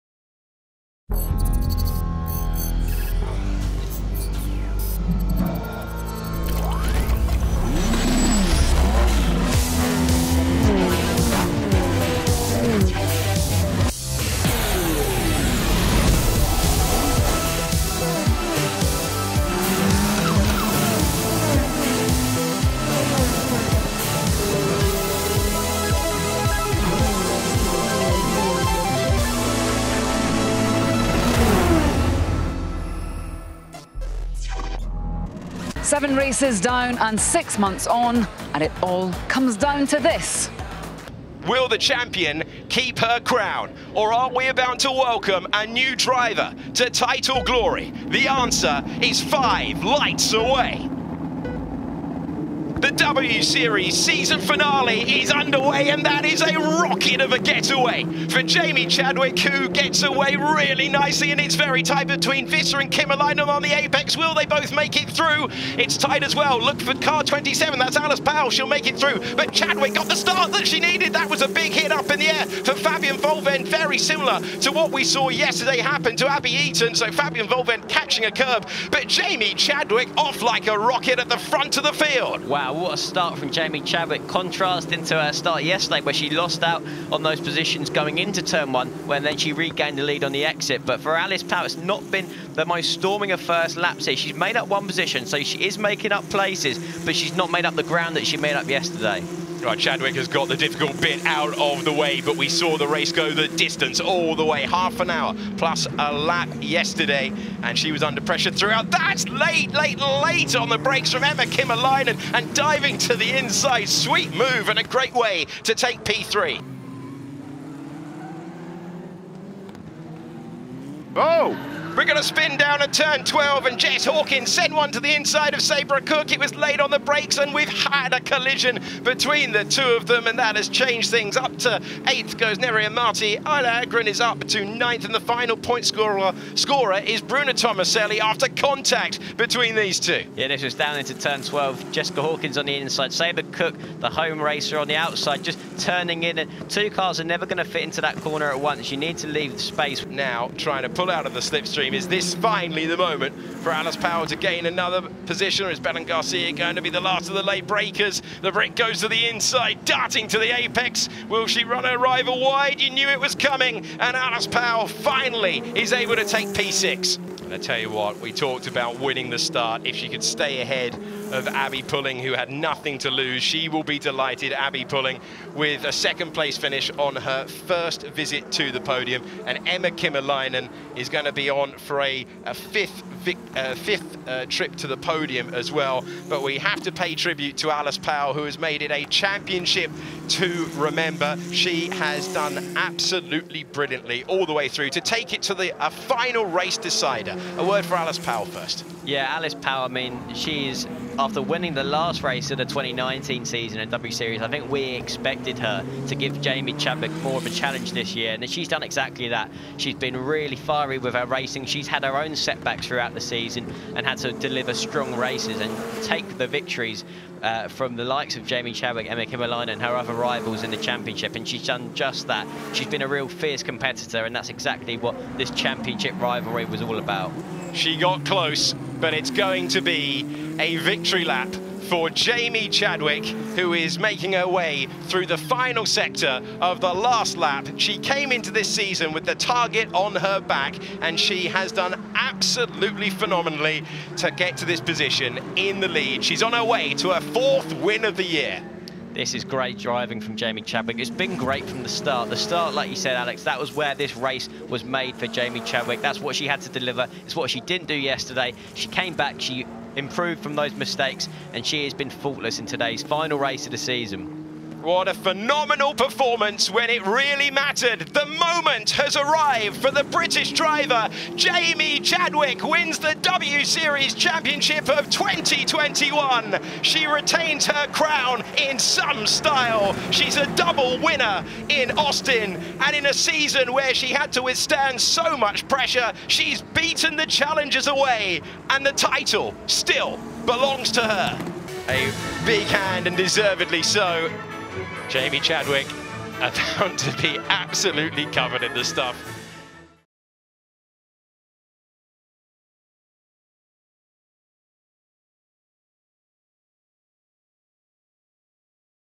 W Series Finale! Race 2 Highlights | 2021 United States Grand Prix
It's a championship showdown in Texas! As Jamie Chadwick looks to take her second W Series crown, with only Alice Powell the driver who can stop her...